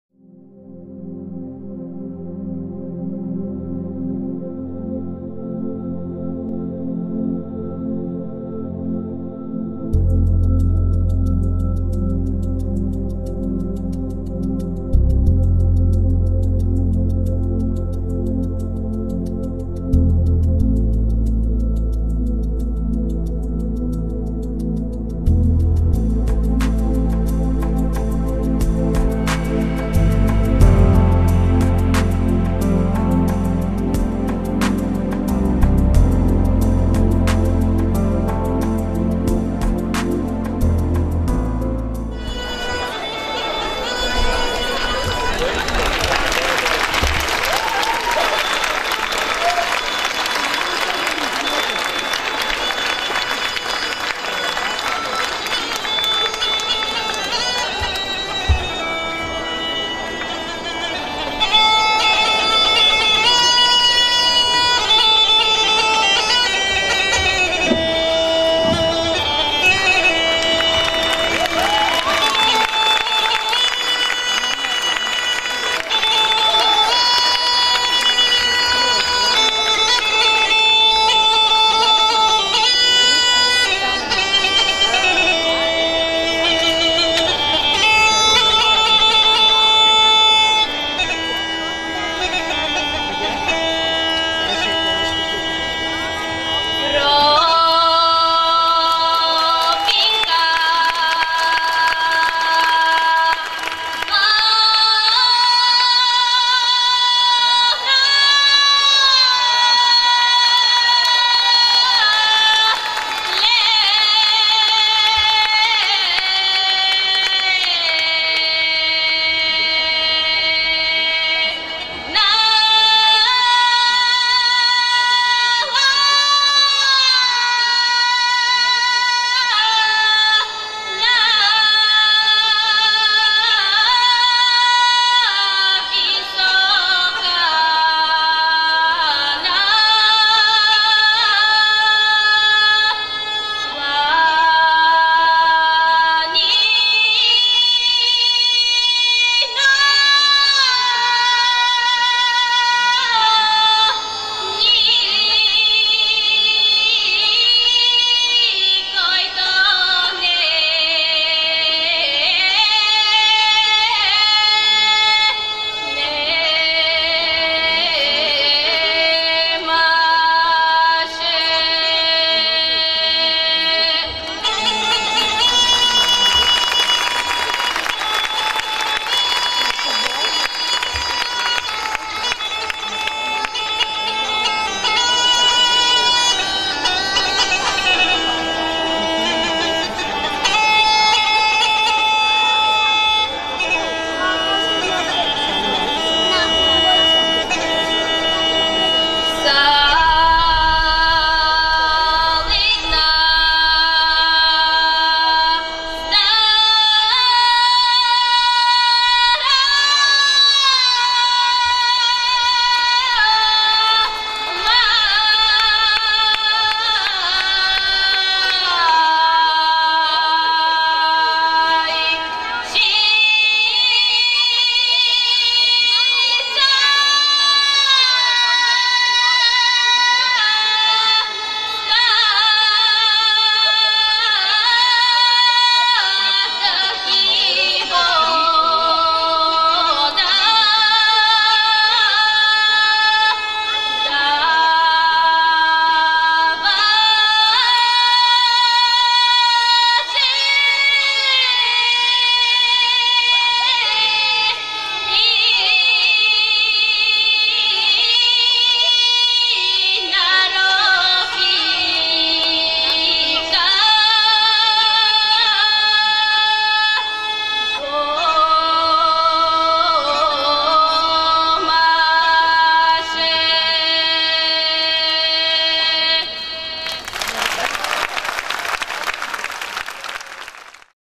"НА МЕГДАНА КРАЙ МОРЕТО" :ПЕТЯ ПАНЕВА 06.09.2014
Национален конкурс за фолклорни танци и хора на любителски  клубове, Организиран от НЧ "Просвета 1927" Варна